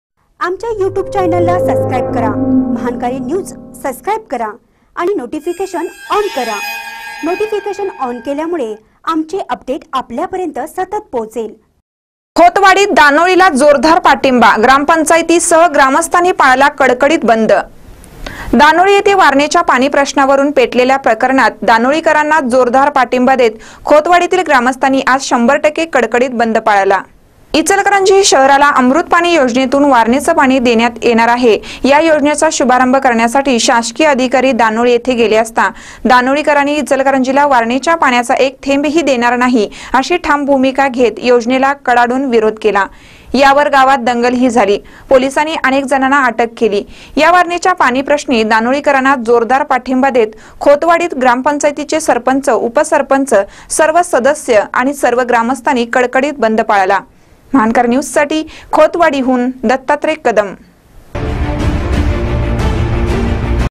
खोतवाडीत दानोळीला जोरदार पाठींबा
खोतवाडीत दानोळीला जोरदार पाठींबा : ग्रामपंचायतीसह ग्रामस्थांनी पाळला कडकडीत बंद